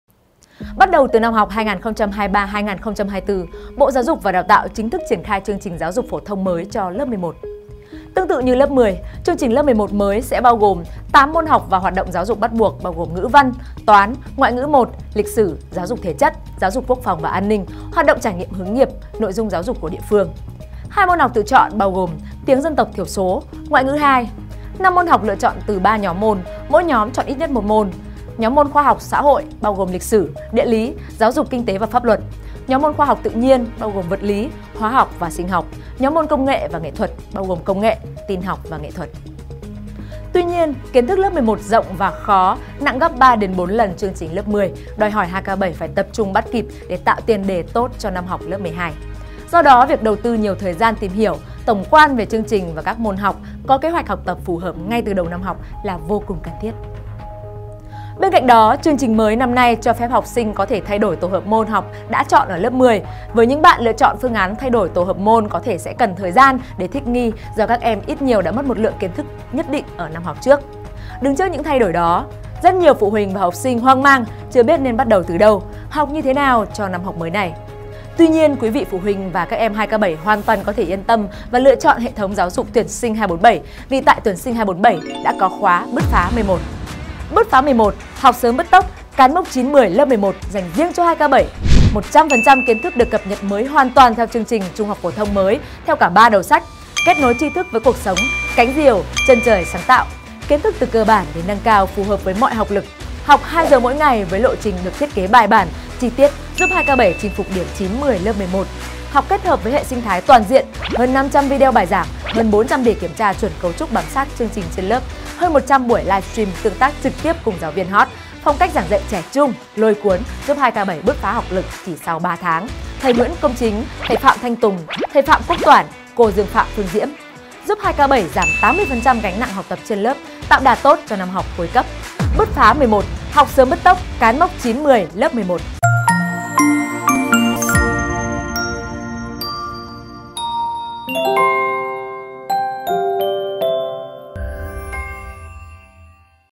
Chương trình lớp 11 mới sẽ được triển khai ra sao?
⚠ Kiến thức lớp 11 rộng và khó, nặng gấp 3-4 lần chương trình lớp 10
⚠ Chương trình lớp 11 mới năm nay cho phép học sinh có thể thay đổi tổ hợp môn học đã chọn ở lớp 10.
Đứng trước những thay đổi đó, các em nên có kế hoạch học tập từ sớm và cân nhắc thật kĩ lưỡng nếu có ý định thay đổi tổ hợp môn.